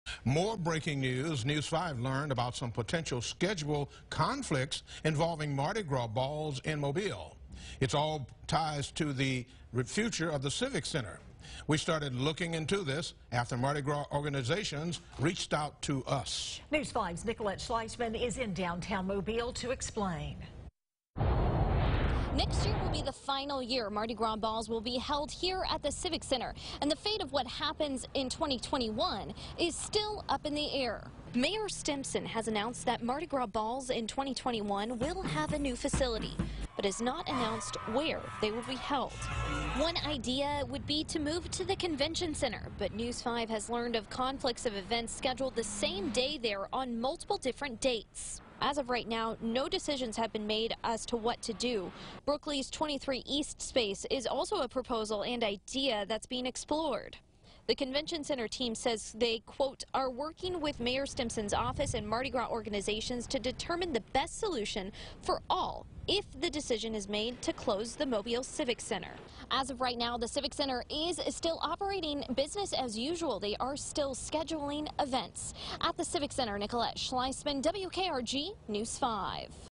VIDEO: Future of Mardi Gras balls at Civic Center up in the air
News 5 has gotten tips about conflicts with Mardi Gras organizations and balls after the Civic Center stops hosting balls.